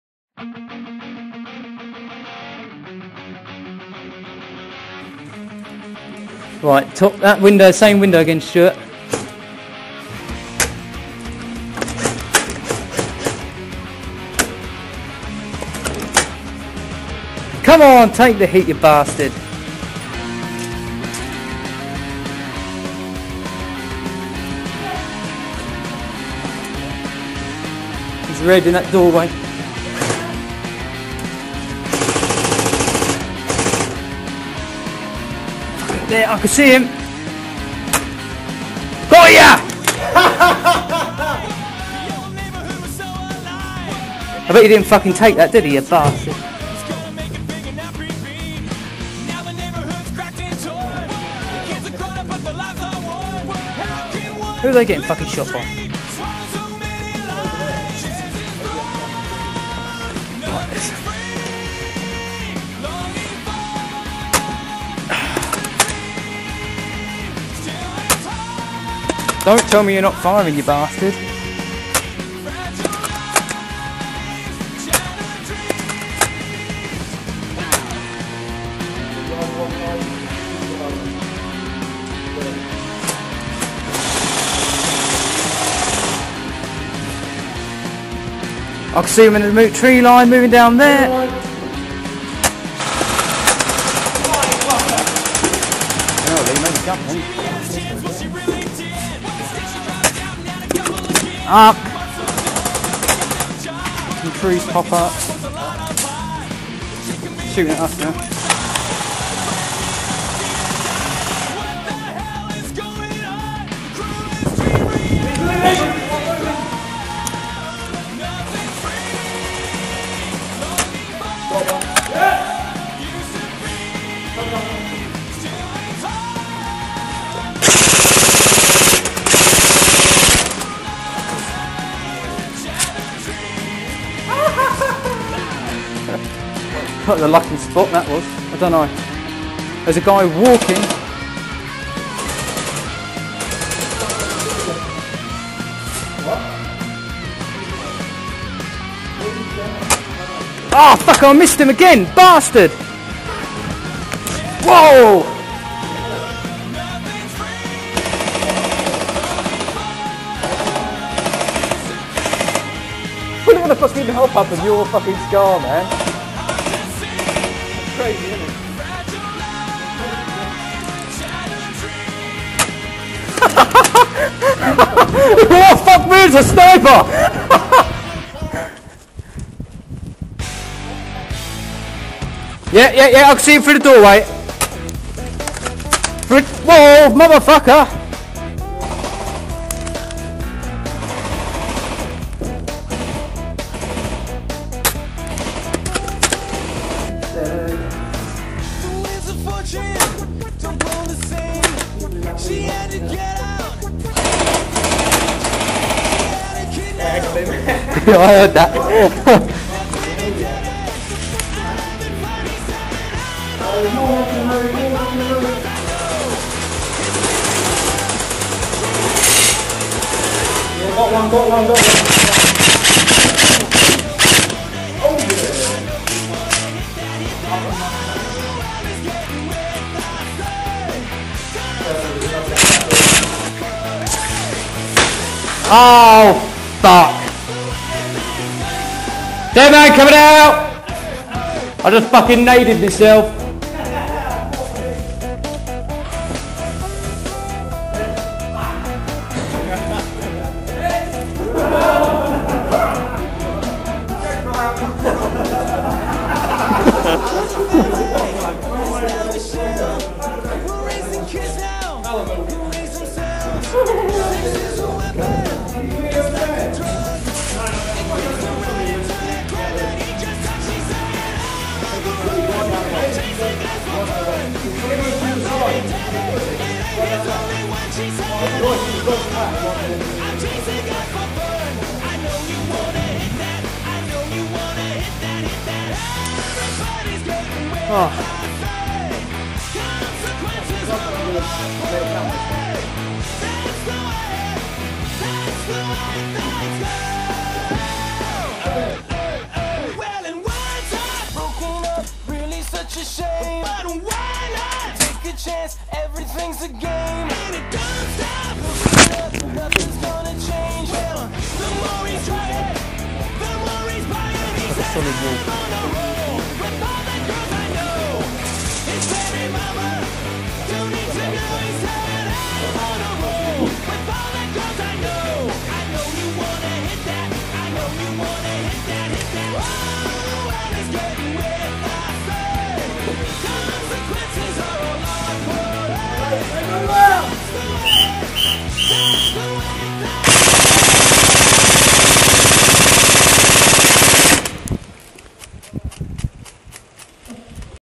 Urban Assault Airsoft - Oct 25th 2008
Footage from Urban Assault Airsoft - Oct 25th 2008, Red Team.

Footage from my Archos 604 Headcam.